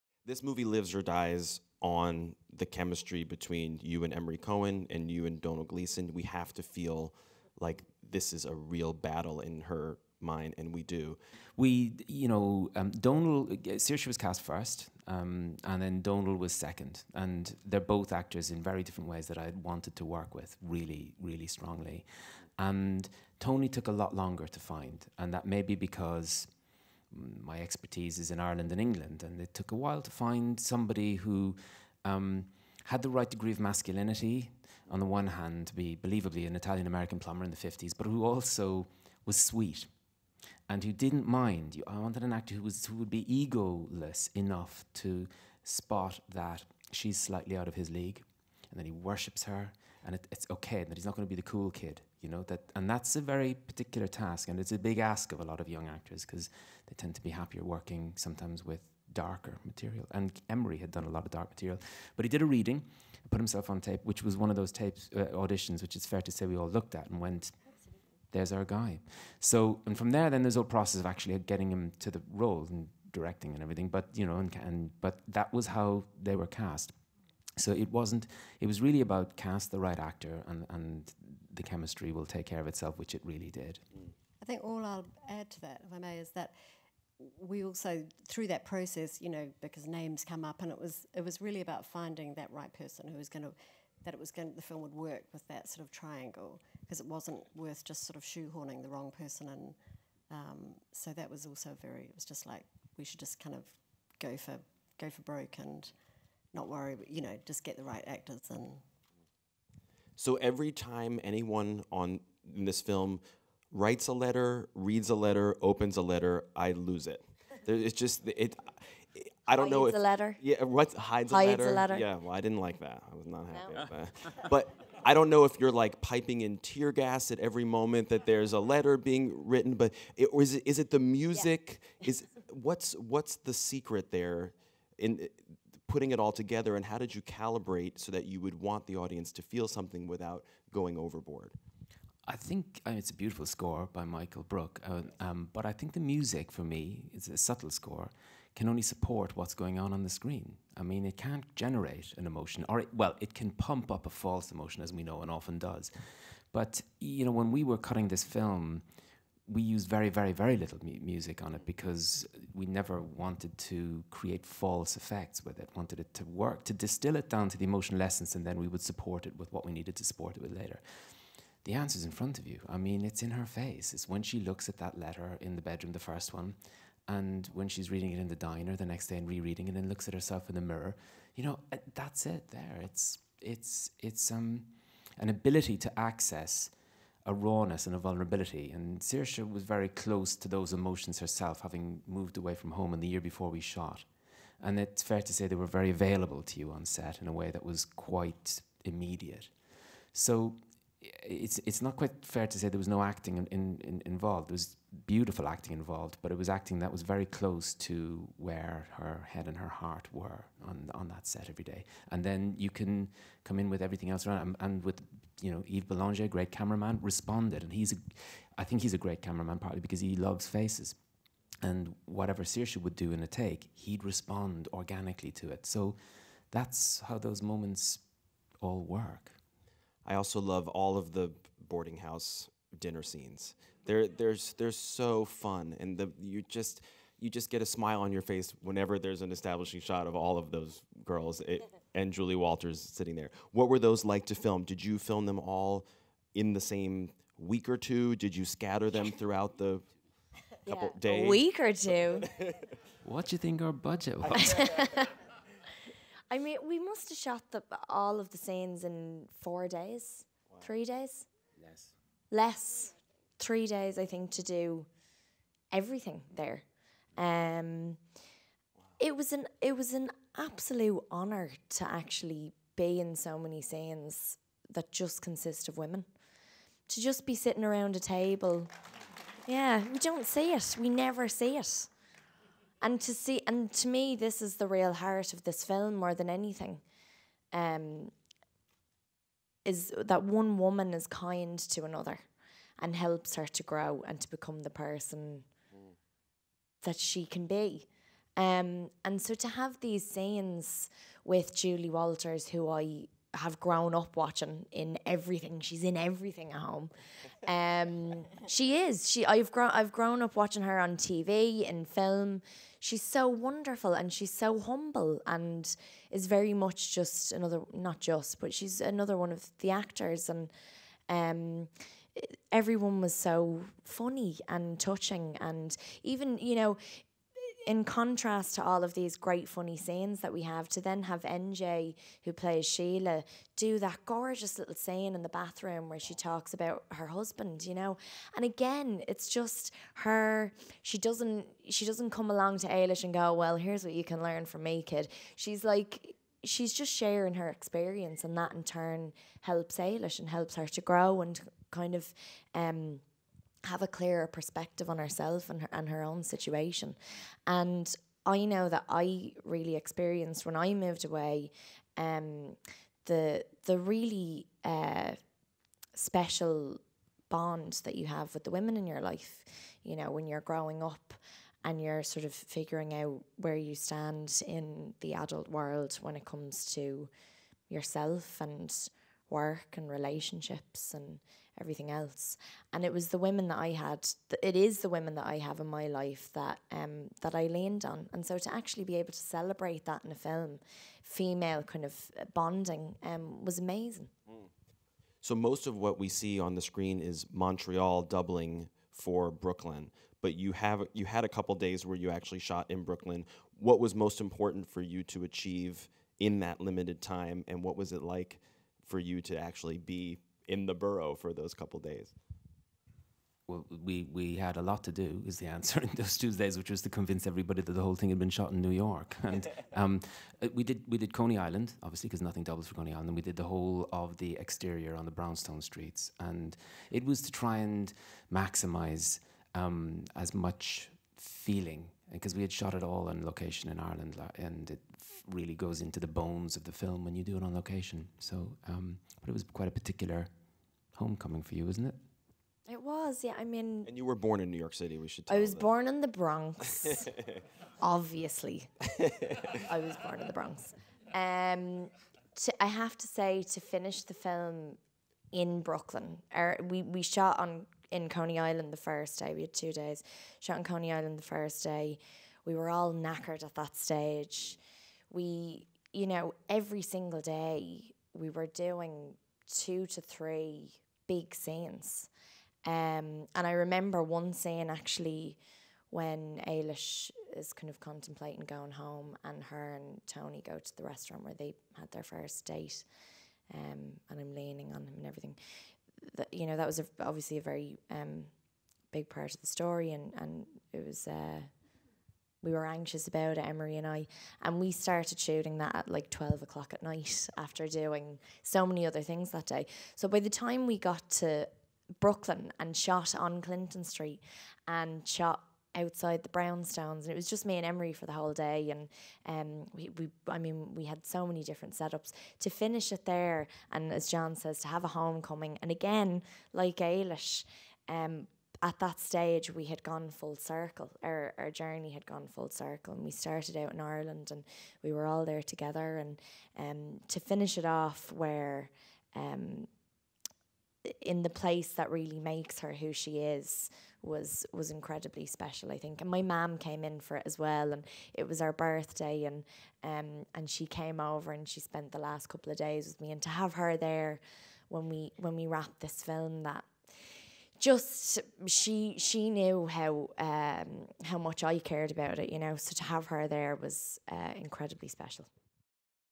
Academy Conversations: Brooklyn
Brooklyn discussion with actor Saoirse Ronan, director John Crowley and producer Finola Dwyer on November 7, 2015 at the Samuel Goldwyn Theater.